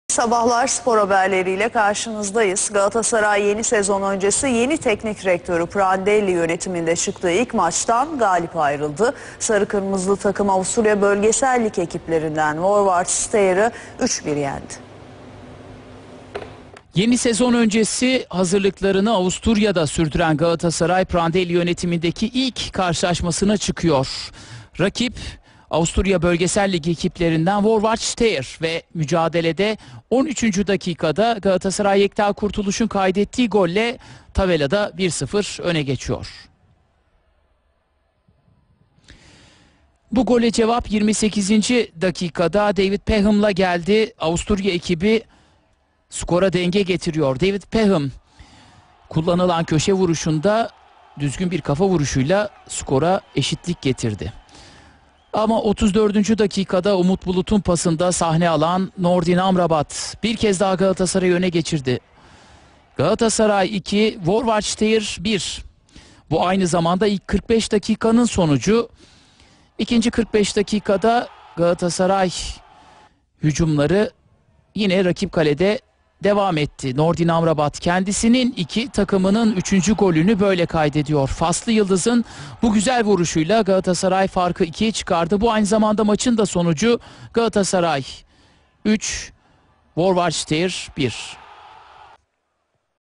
Vorwarts Steyr 1 - 3 Galatasaray.. (-Hazırlık Maçı-) 21.07.2014 ᴴᴰ
Vorwarts Steyr 1 - 3 Galatasaray.. (-Hazırlık Maçı-) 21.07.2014
GOLLER - DK.28 David Peham // DK.14 Yekta Kurtuluş, DK.34 Nordin Amrabat, DK.64 Nordin Amrabat..#